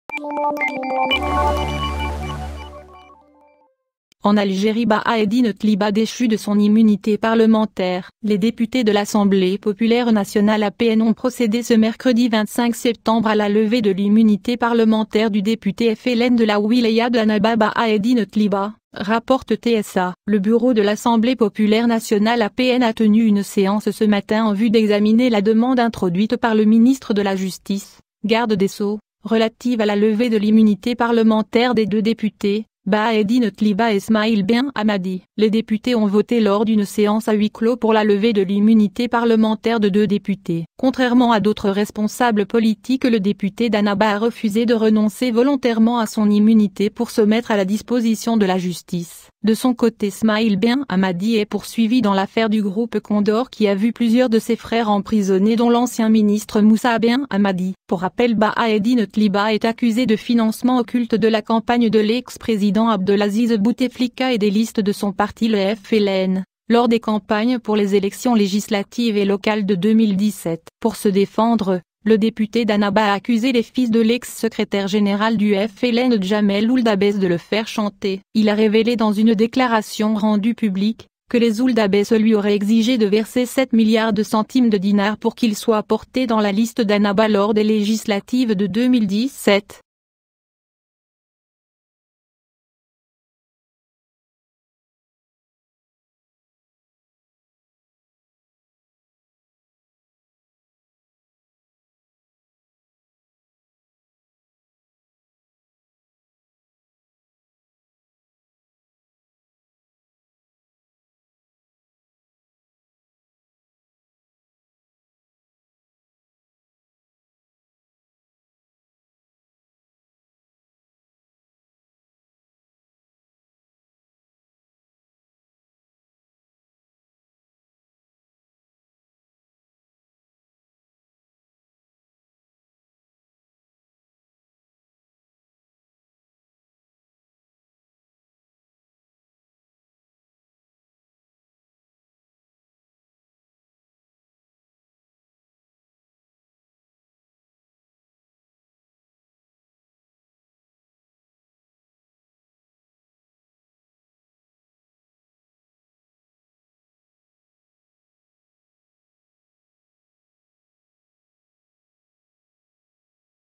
levée de l’immunité parlementaire du député FLN de la wilaya de Annaba Baha Eddine Tliba
Algérie : Baha Eddine Tliba déchu de son immunité parlementaire

Les députés de l’Assemblée populaire nationale (APN) ont procédé ce mercredi 25 septembre à la levée de l’immunité parlementaire du député FLN de la wilaya de Annaba Baha Eddine Tliba, rapporte TSA.

Le bureau de l’Assemblée populaire nationale (APN) a tenu une séance, ce matin, en vue d’examiner la demande introduite par le ministre de la Justice, Garde des Sceaux, relative à la levée de l’immunité parlementaire des deux députés, Baha-Eddine Tliba et Smaïl Benhamadi. Les députés ont voté lors d’une séance à huis clos pour la levée de l’immunité parlementaire des deux députés.

Contrairement à d’autres responsables politiques, le député d’Annaba a refusé de renoncer volontairement à son immunité pour se mettre à la disposition de la justice. De son côté, Smaïl Benhamadi est poursuivi dans l’affaire du groupe Condor, qui a vu plusieurs de ses frères emprisonnés, dont l’ancien ministre Moussa Banhamadi.

Pour rappel, Baha Eddine Tliba est accusé de « financement occulte » de la campagne de l’ex-président Abdelaziz Bouteflika et des listes de son parti le FLN, lors des campagnes pour les élections législatives et locales de 2017.

Pour se défendre, le député d’Annaba a accusé les fils de l’ex-secrétaire général du FLN Djamel Ould Abbes de le faire « chanter ». Il a révélé dans une déclaration rendue publique, que les Ould Abbes lui auraient exigé de verser 7 milliards de centimes de dinars pour qu’il soit porté dans la liste d’Annaba lors des législatives de 2017.